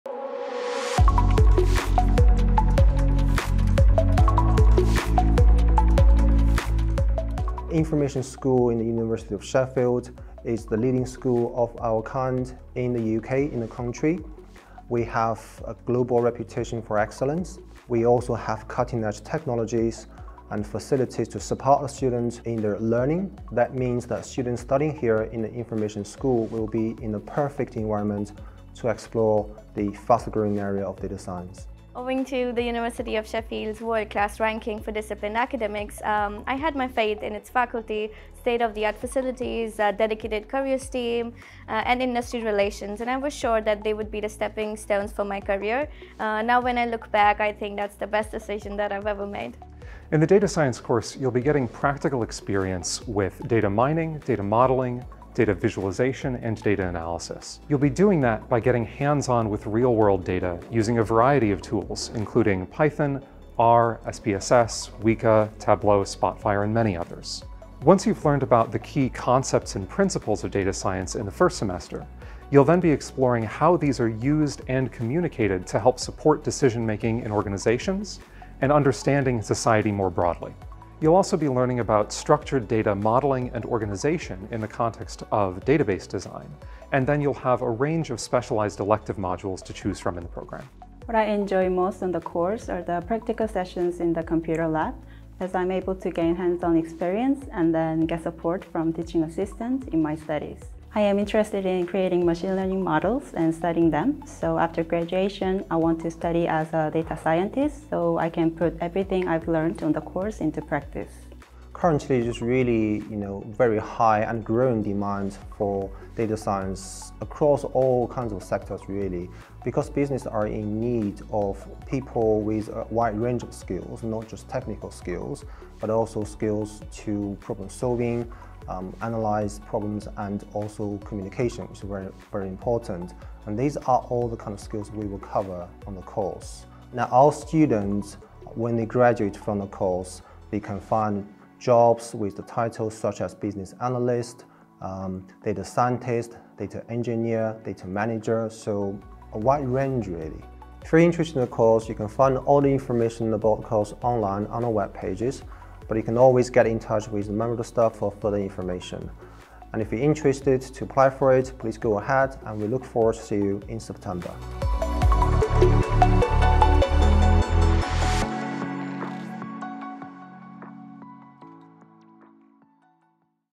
MSc Data Science at the University of Sheffield
Find out why studying MSc Data Science at the University of Sheffield Information School might be right for you!